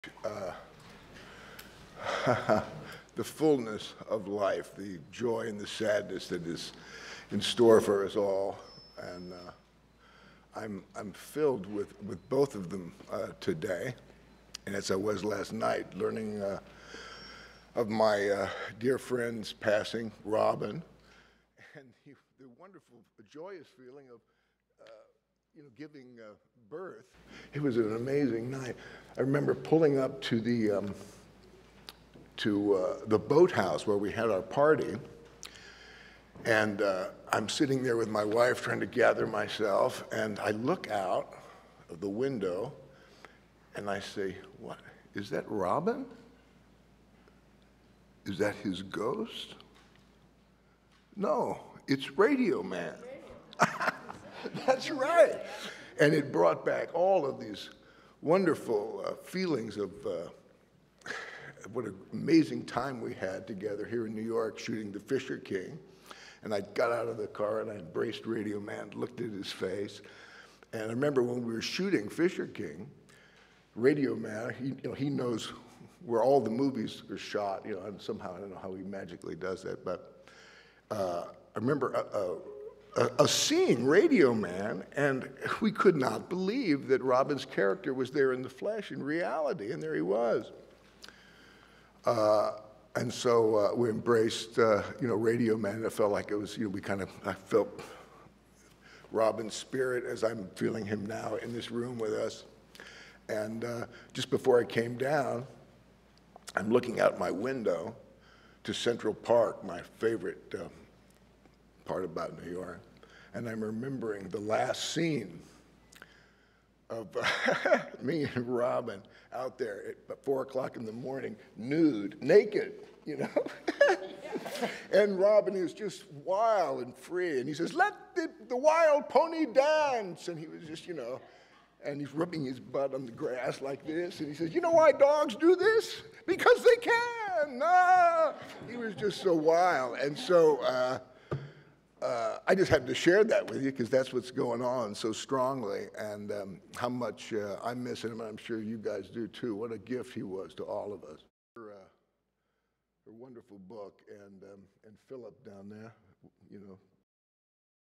Jeff Bridges reacts to the death of Robin Williams at a press conference for 'The Giver'
(12 Aug 2014) JEFF BRIDGES MOURNS  FRIEND AND SAYS HE THOUGHT HE SAW ROBIN WILLIAMS' GHOST AFTER LEARNING OF HIS DEATH
Jeff Bridges would not talk to the media about his friend Robin Williams  on Monday (11 AUG. 2014) evening at the red carpet premiere of his new film "The Giver."
 But Tuesday morning at a press conference for the film he  tearfully acknowledged the death of his "dear friend" and told the audience how only hours after learning of his Williams' suicide he thought he saw his ghost.
"I remember pulling up to the boathouse where we had our party (for 'The Giver') and I'm sitting there with my wife trying to gather myself and I look out the window and I say, 'Is that Robin? Is that his ghost? No, it's radio man," Bridges told reporters. "And it brought back all of these wonderful feelings of what an amazing time we had together here in New York shooting 'The Fisher King' and got out of the car and I embraced radio man and looked into his face and I remember when we were shooting 'Fisher King ,' radio man, you know,  he knows  where all the movies are shot and somehow he magically does that, but I remember a scene, radio man, and we could not believe that Robin's character was there in the flesh, in reality and there he was. And  so, you know, we embraced radio man and it felt like, I felt Robin's spirit as I'm feeling him now in this room with us."
Radio man is something of an institution in Manhattan  - an elderly gentleman who always seems to have a radio on him and who always seems to know the locations of film shoots.  Celebrities have embraced him over the years.
Bridges , alternating between laughter and tears, went on to recall an evening he spent with Williams in Central Park during the shooting of the 'The Fisher King.' He
"Just before I came down I'm looking out my window to Central Park, my favorite part about New York, and I'm remembering the last scene  of me and Robin out there at four o'clock in the morning nude, naked,  and  Robin is just wild and free and he's just 'Let the wild pony dance,' he's just you know, and he's rubbing his butt on the grass and he says 'You know why dogs do this - because they can!' and we were just so wild and so - I just had to share that with you, 'cause that's what's going on so strongly and how much I miss him and I'm sure you guys do too. What a gift he was to all of us."